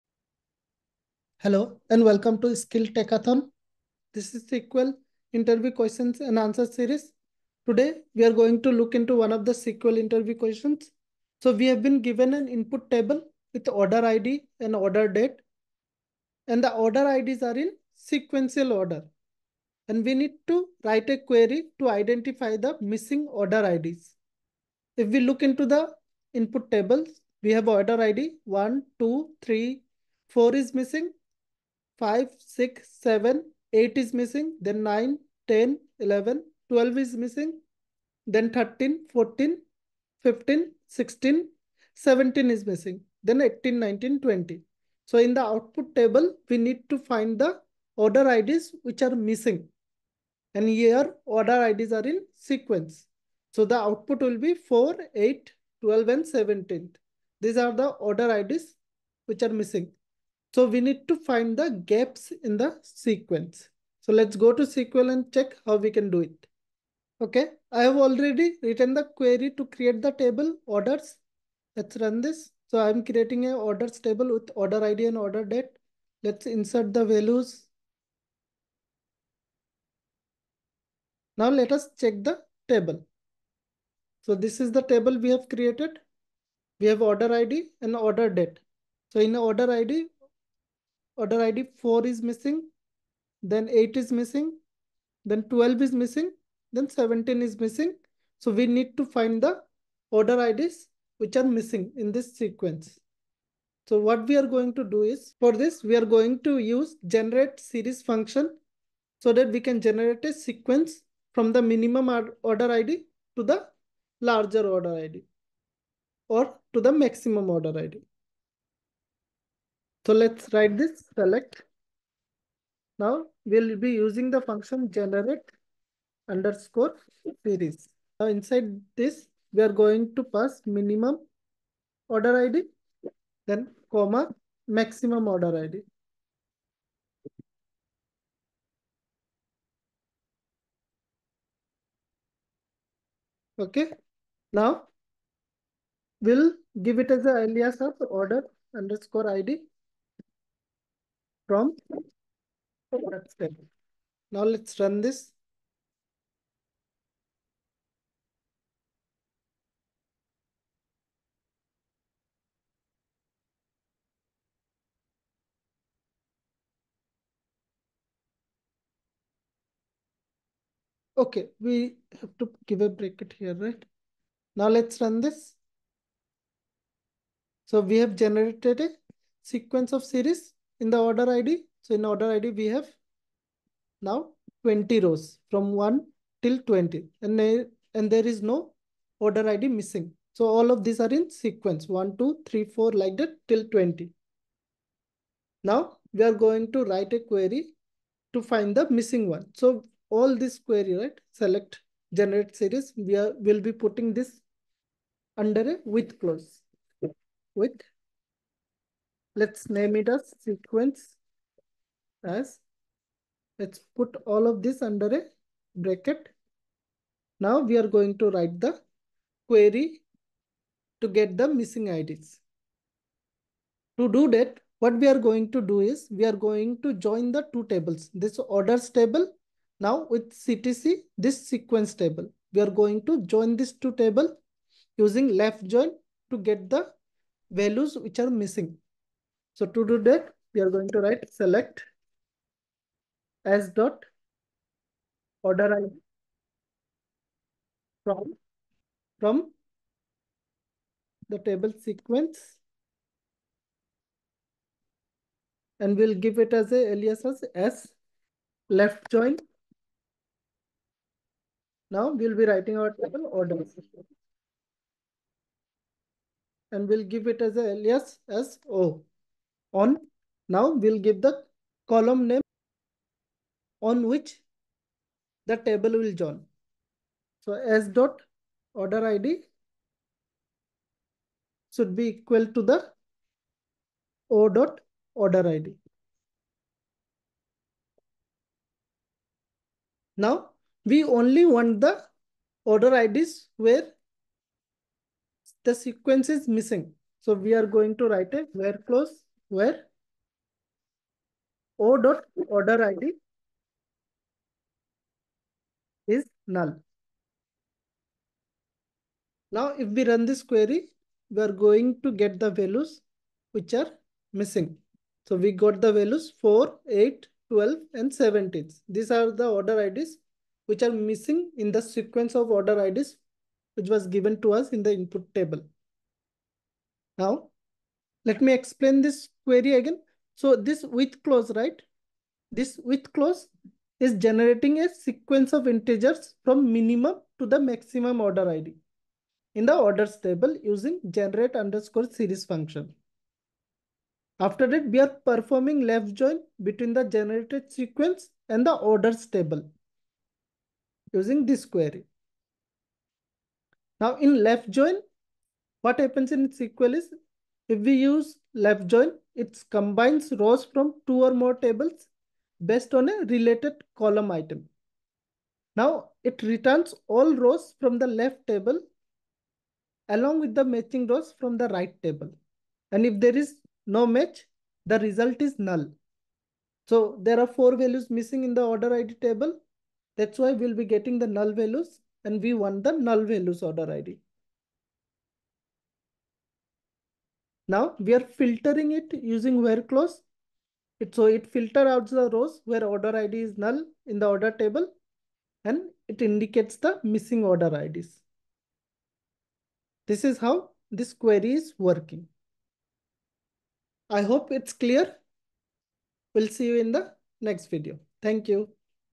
UST Global SQL Interview Question 2024 | Identifying Gaps in a Sequence
In this video, I have discussed how to solve the SQL Interview Questions:
Given a table orders with columns order_id (sequential integers), order_date, write a query to identify the missing order IDs.

-- Query to create table:
CREATE TABLE orders (
    order_id INT PRIMARY KEY,
    order_date DATE

-- Insert Data
INSERT INTO orders (order_id, order_date) VALUES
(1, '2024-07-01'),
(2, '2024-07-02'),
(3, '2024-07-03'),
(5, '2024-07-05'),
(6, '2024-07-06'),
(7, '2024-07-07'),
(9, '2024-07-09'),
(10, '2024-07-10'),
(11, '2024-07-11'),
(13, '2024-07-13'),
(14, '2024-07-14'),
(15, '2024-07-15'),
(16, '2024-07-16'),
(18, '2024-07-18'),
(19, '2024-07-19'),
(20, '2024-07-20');